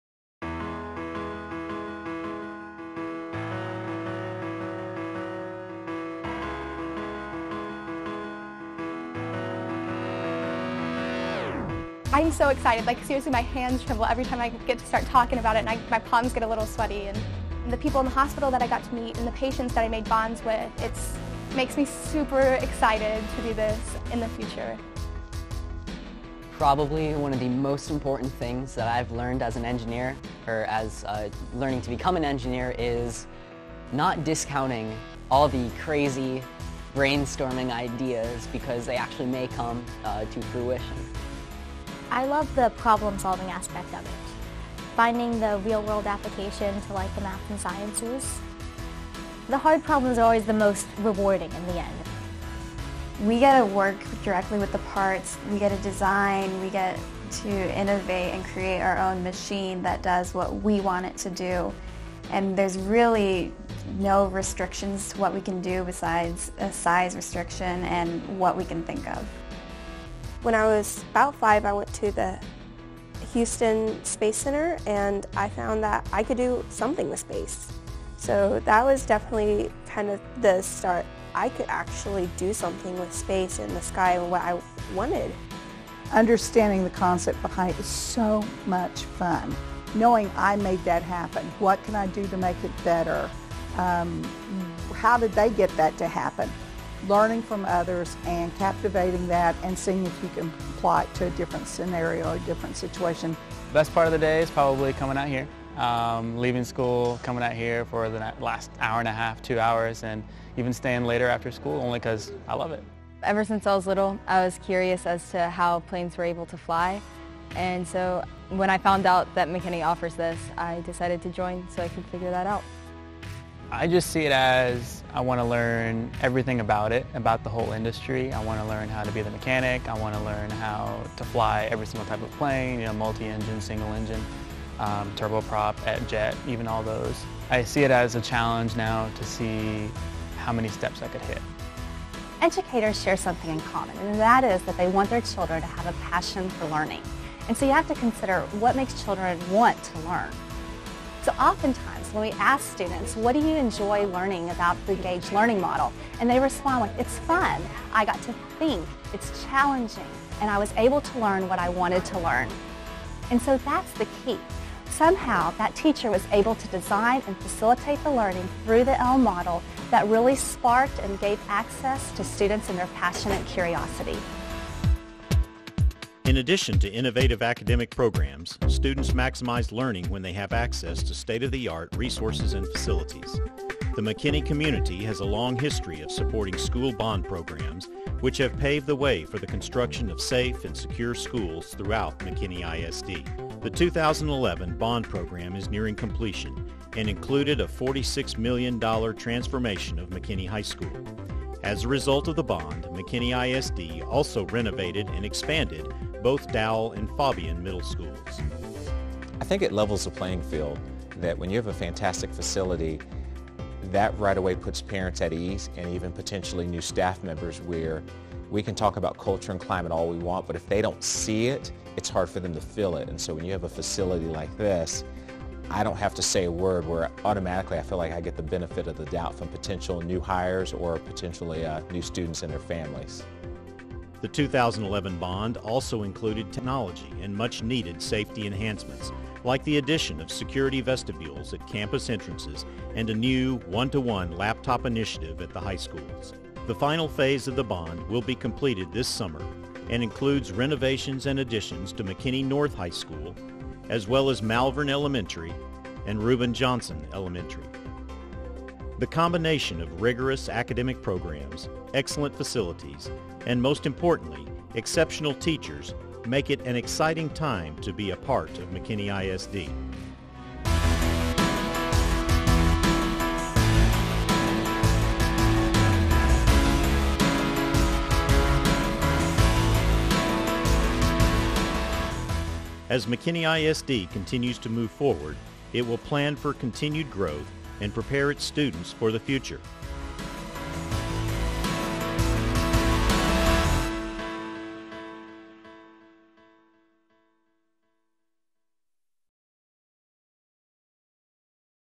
2015 State of the City - McKinney ISD
Video presentation from McKinney ISD at the 2015 State of the City.